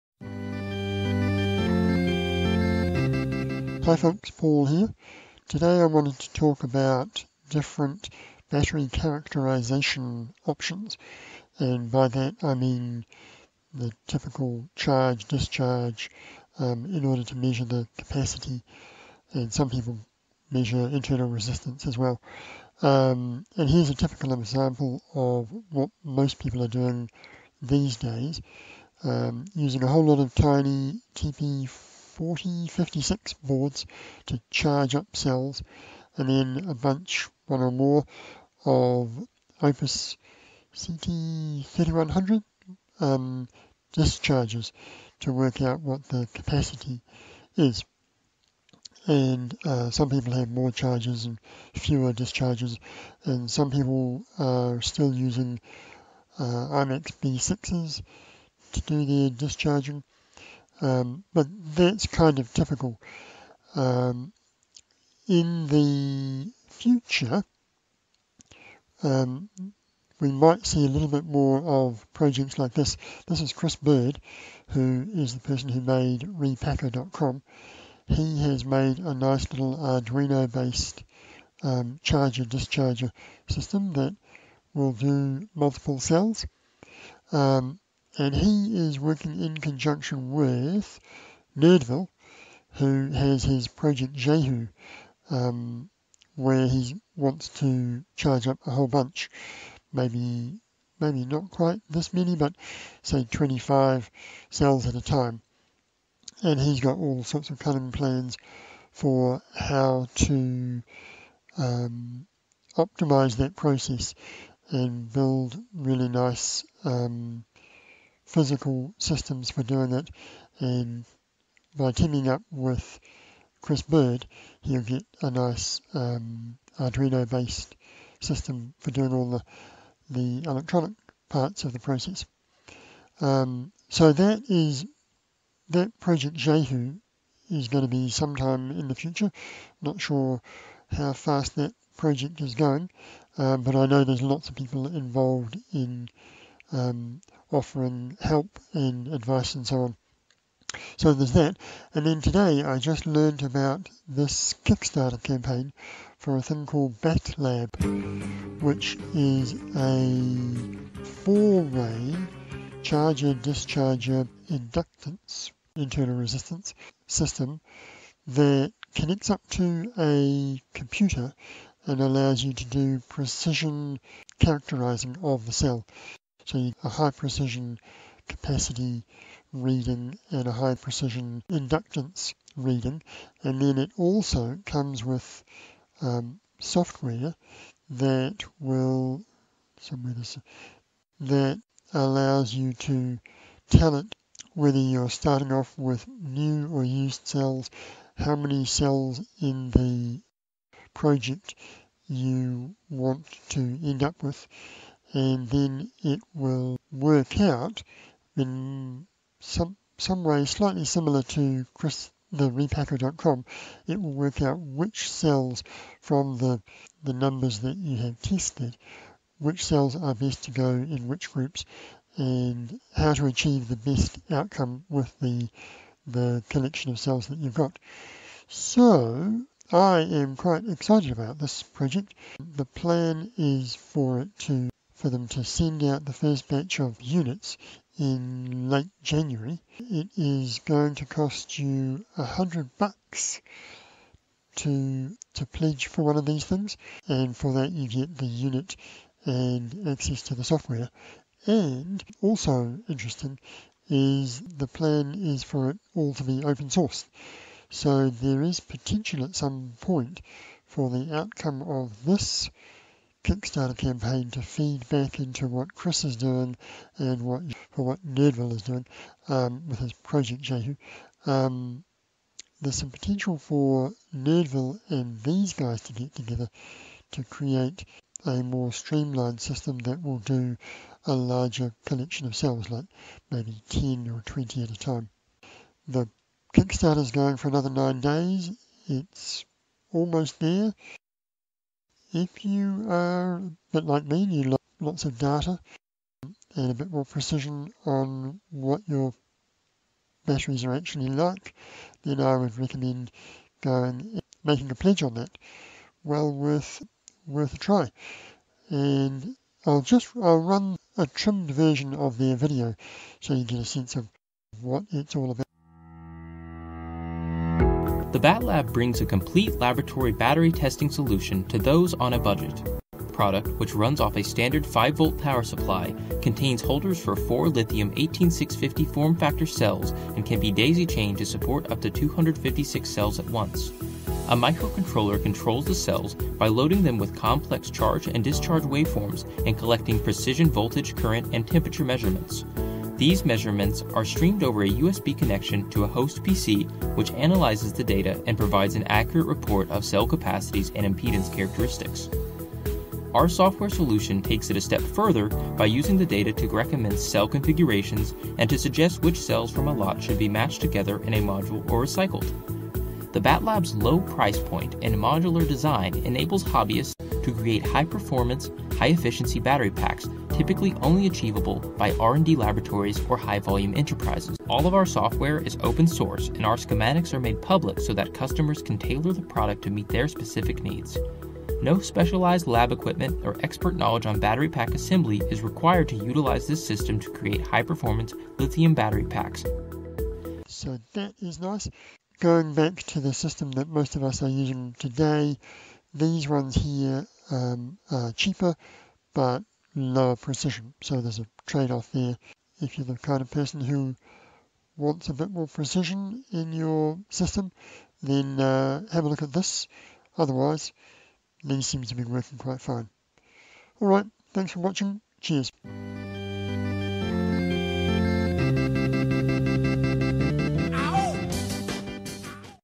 Laptop Powerwall 88 - battery characterization options
Looking at several ways to test the quality of cells.